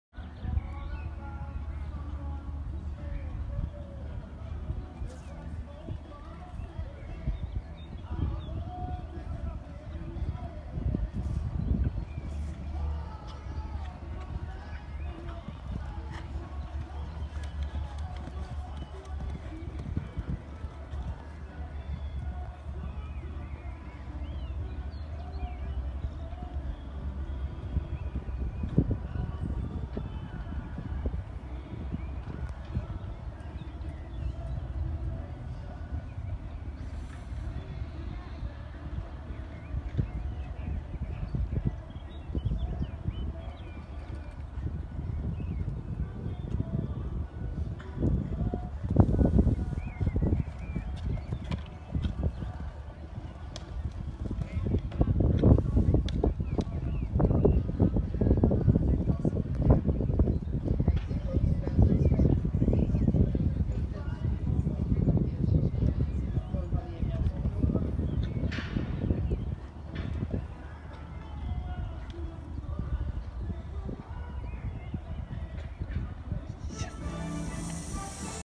Elien CSI2* 1.45m Grand Prix Sentower Park 11.5.2019
Elien (mare born 2009 by Carambole x Mermus R) CSI2* 1.45m Grand Prix (first round) Sentower Park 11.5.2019